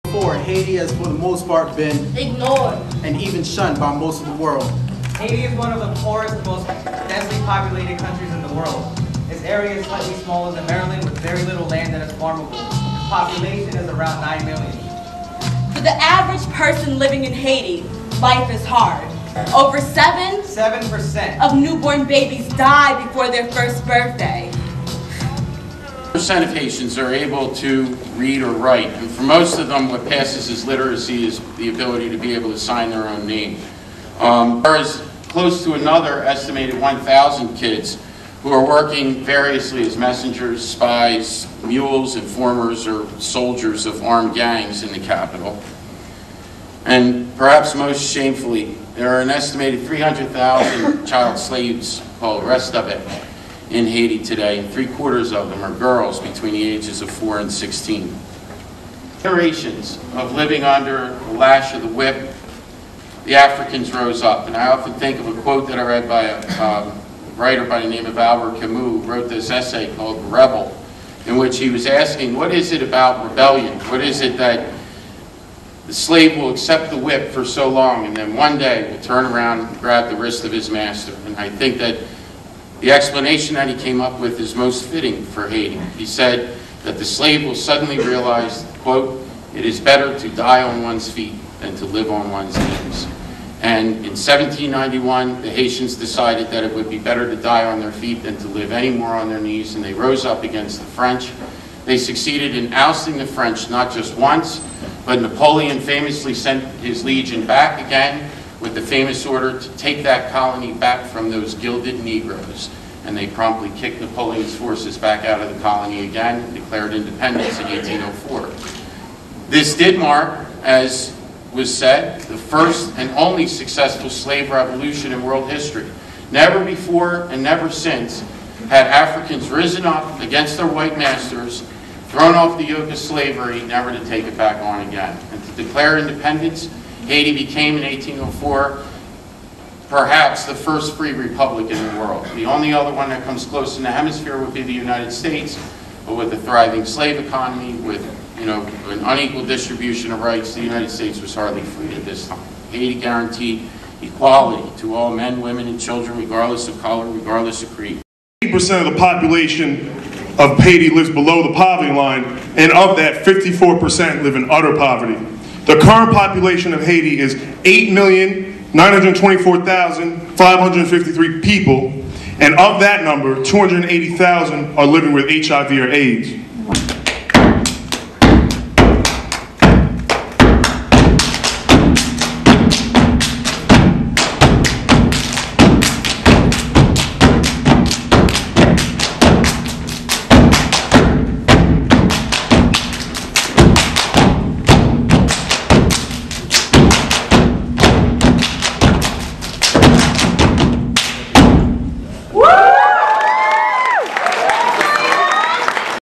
Highlights from Rock 4 Haiti
Rock 4 Haiti, a benefit event to raise awareness about the economic and social problems in Haiti, was held Fri., March 20.
Student Union, the event featured spoken word by students as well as hip hop artist Blitz the Ambassador, LU's Finest Step Team, and Muhlenberg College Assistant Professor of Anthropology J. Christopher Kovats-Bernat.